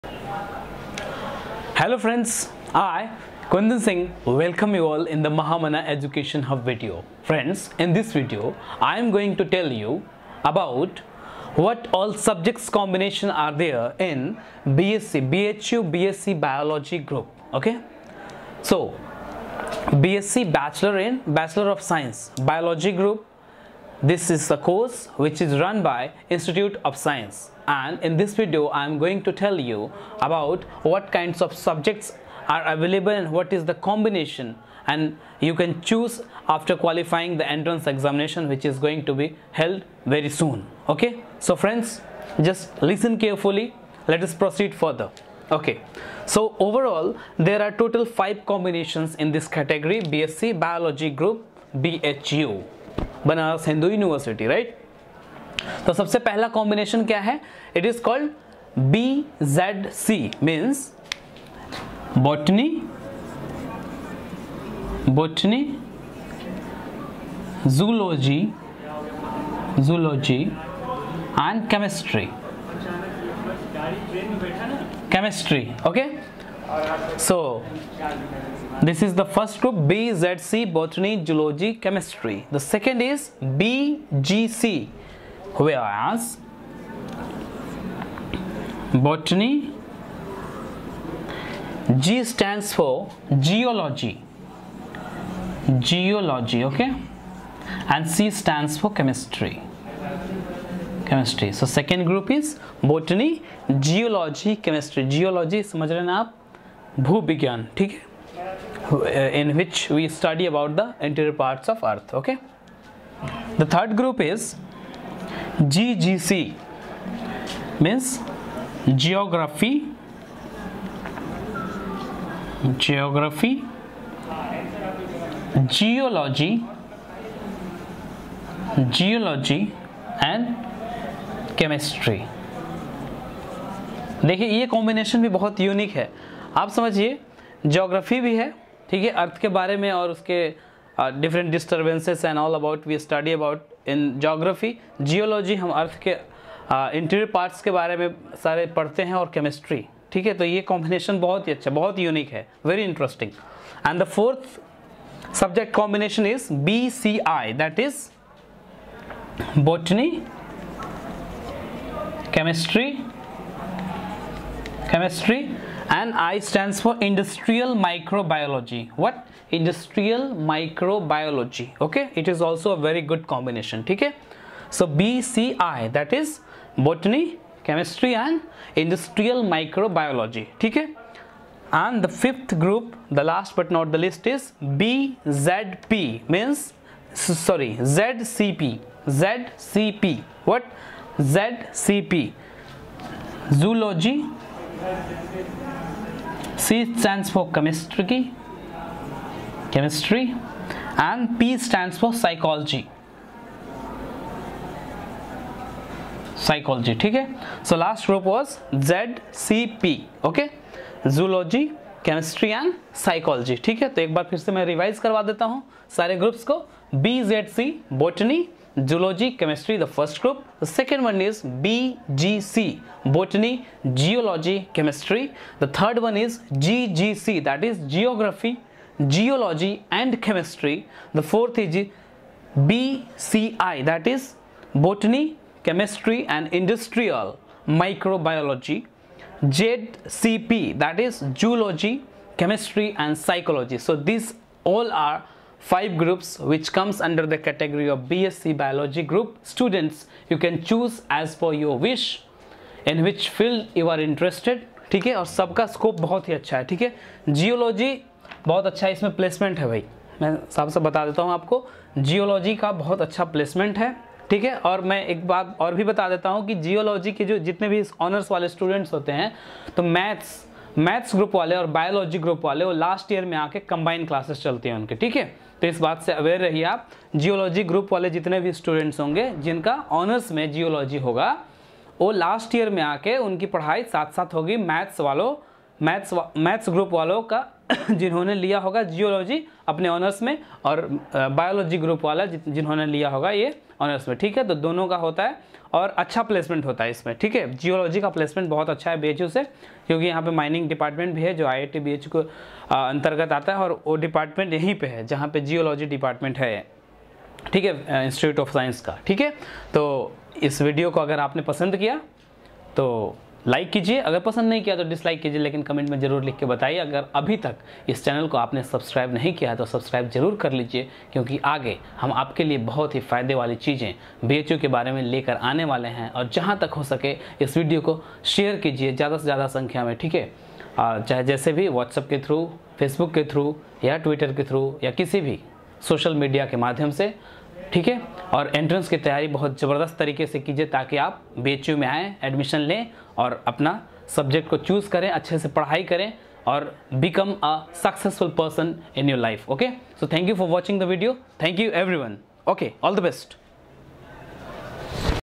BHU BSc BIOLOGY GROUP SUBJECT COUNSELING & COMBINATION..FULL DETAIL
THIS  video  is  useful  to  counsel  the  students  who  are  going  to  appear  for  BSc  Biology  group